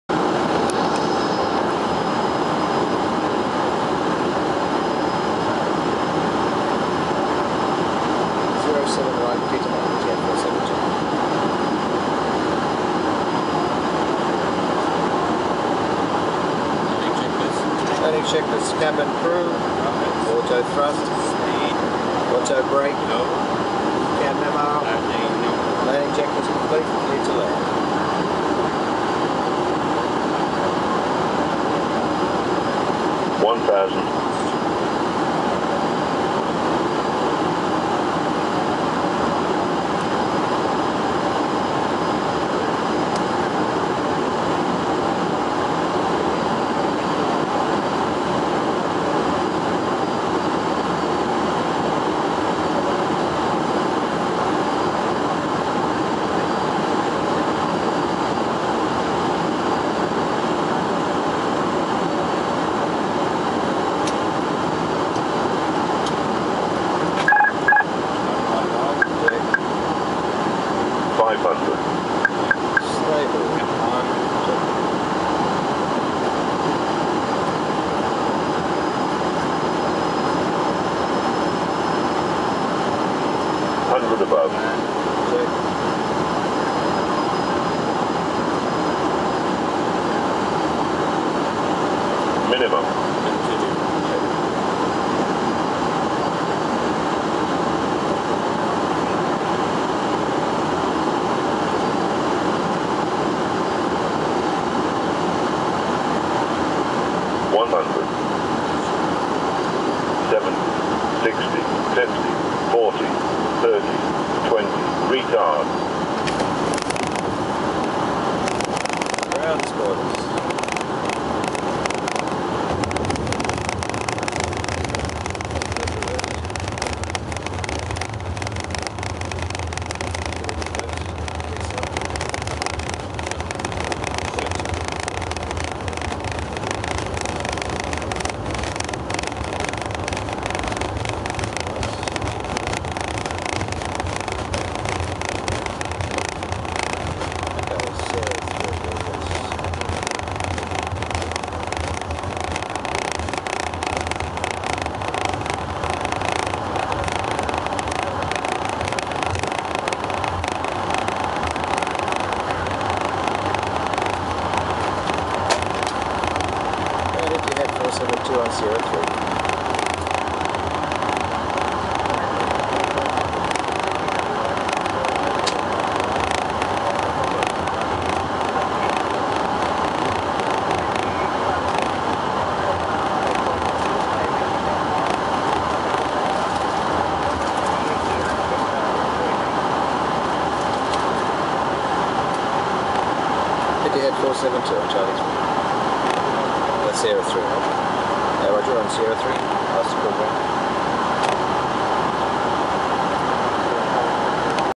A330 Day Appr & Ldg RWY 07R Jakarta International Airport - Cockpit View
A330 - Video clip of a day approach and landing on runway 07R at  Jakarta International airport as seen from the flightdeck. From 1500ft to taxiway turnoff this video puts you in the pointy end of an Airbus A330 during this critical and most interesting phase of flight.

Visibility 8000m Haze

Filmed 16Aug11 with Sony Cybershot W360 camera.